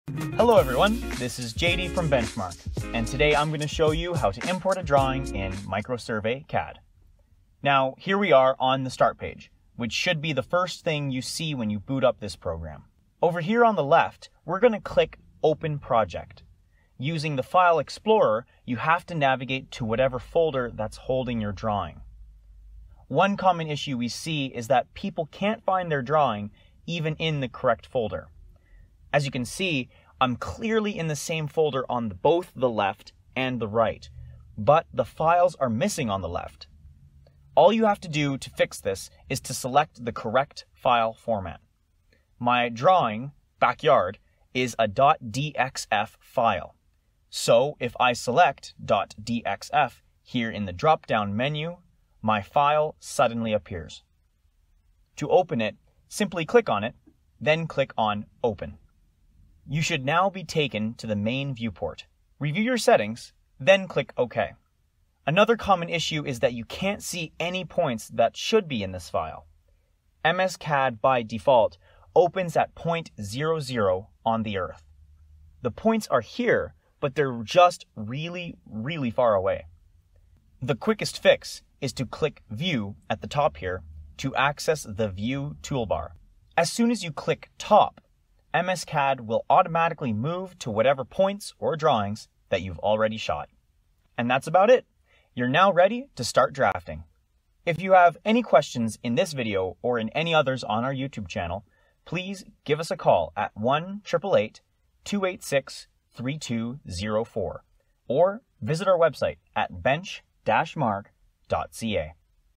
MicroSurvey CAD How to: Importing a .dxf File | Bench Mark
In this video, JD from Bench Mark shows you how to import a .dxf file into MicroSurvey CAD as well as a couple of issues people often experience during this process.